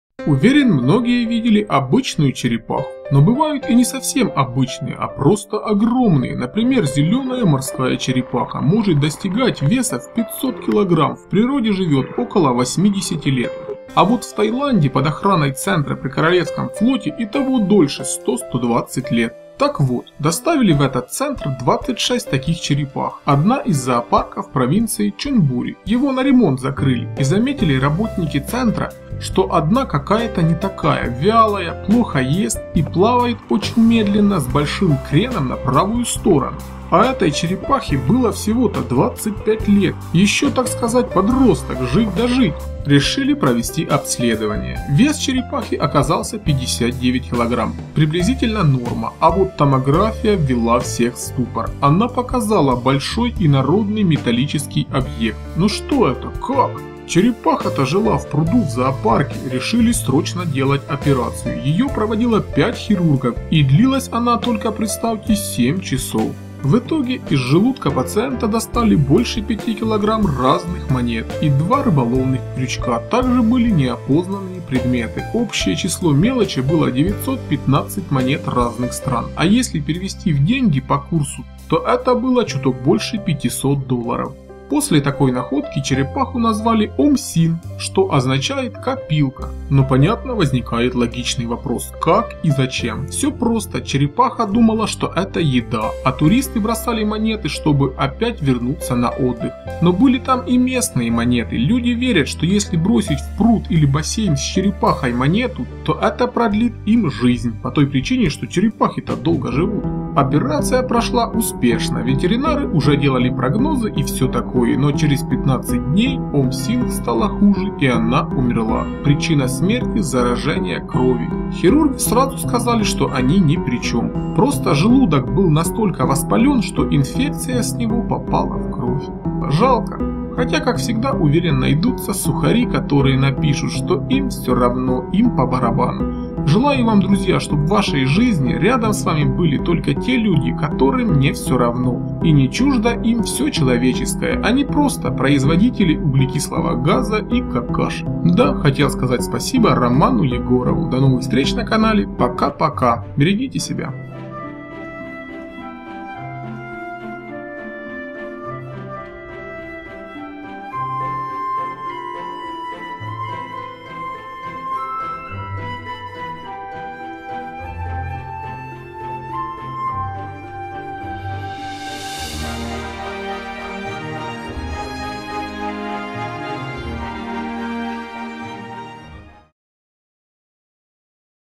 ЧТО МОЖЕТ БЫТЬ В ПРОСТОЙ ЧЕРЕПАХЕ И КАК ТАКОЕ ВОЗМОЖНО?
Как суеверия и человеческая халатность привели к непоправимому  .И только когда сделали томографию все стало ясно.Трагическая история которая произошла не так давно.
В поисках Золота и Старины с Дмитрием.